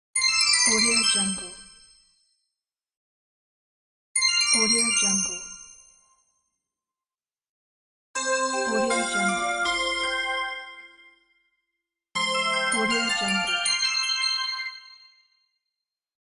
Sound - 4 Happy Eureka Sound Effects | AudioJungle
Download Sound - 4 Happy Eureka Sound Effects | AudioJungle

The following are included in the bundle; the preview plays the following sounds in this order:
1. 0:04
2. 0:04
3. 0:04
4. 0:04
Great for a commercial or a game!